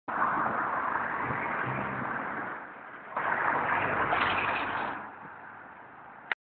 Sam Fairhurst - Feeble Grind Street!
Video uploaded from my Blackberry 8250.

Sam Fairhurst Shredding some street ;D
Expect a full street edit from us soon!